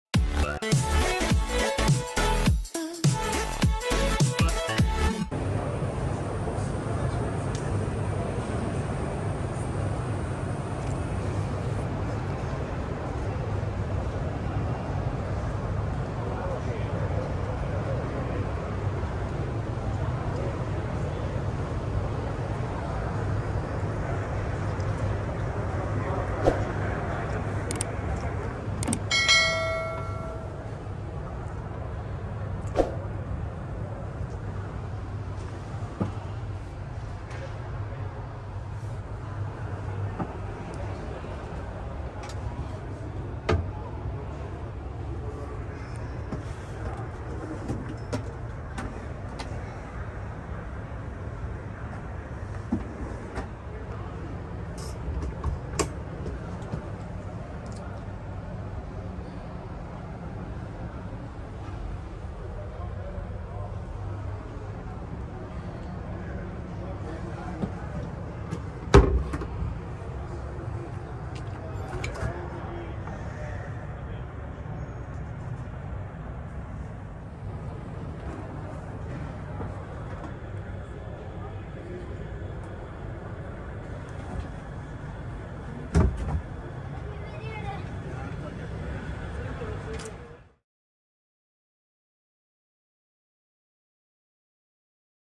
Autostar Performance P680LJ - Modello 2023...Diamo Una Sbirciata !!!
Buongiorno a tutti e benvenuti finalmente al Salone del camper 2022 ...
Come promesso, per tutta la durata della fiera lo staff Meglioli Camper vi proporrà qui sul canale brevi video attraversi i quali potrete salire a bordo dei nostri fantastici veicoli Benimar e Autostar ...
Speriamo che questa iniziativa sia apprezzata soprattutto da chi di voi non riuscirà purtropppo a venire a trovarci in fiera !
Buona visione !
Un saluto dallo staff Meglioli Camper Modena !